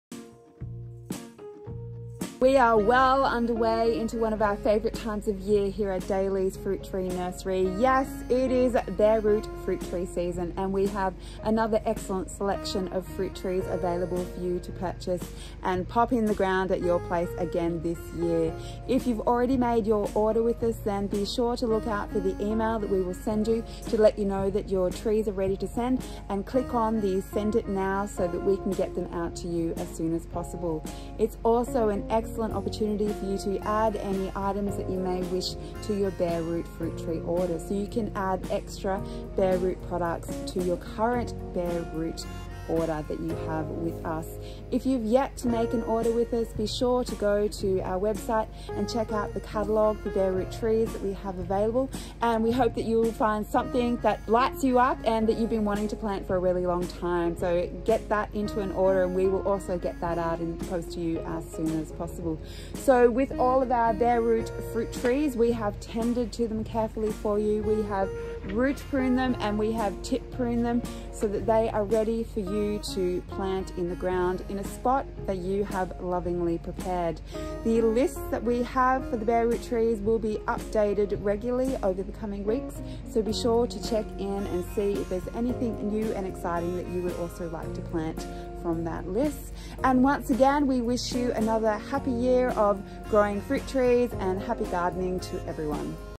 Bare Root Fruit Tree stock levels are growing (Available Once a Year)
Many Bare Root Fruit Trees can only be purchased in Winter. They are bare root because they have no leaves, no pots and are just sent totally dormant looking just like a stick. But with the potential to grow into an amazing fruit tree. They are starting to arrive into Daleys Fruit Tree Nursery. So don't miss the opportunity to get some of these plants.

Bare Root Fruit Trees Ready to be sent.